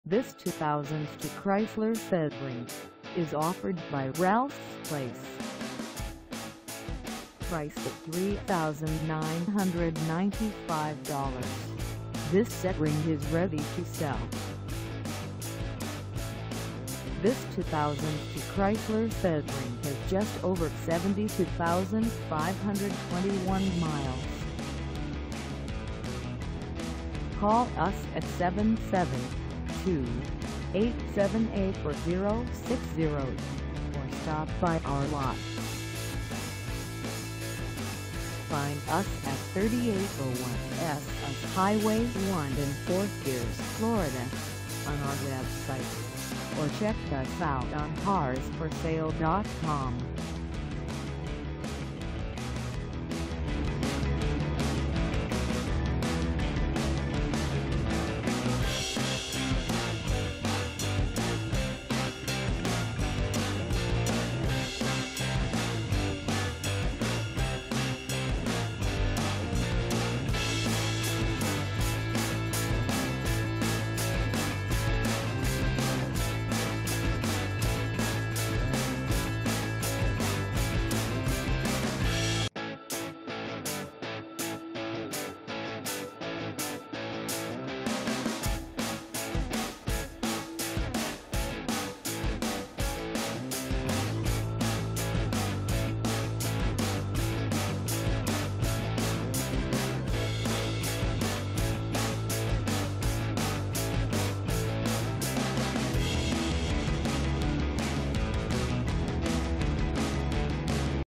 2002 Chrysler Sebring LXi 2dr Convertible for sale in Fort P
This 2002 Chrysler Sebring LXi 2dr Convertible is for sale in Fort Pierce, FL 34982 at Ralph's Place.

Engine: 2.7L V6
Transmission: Automatic 4-Speed
Color: Green
Mileage: 72521
Contact Ralph's Place 3801 S. US Hwy 1 Fort Pierce, FL 34982 to test drive this 2002 Chrysler Sebring LXi 2dr Convertible today.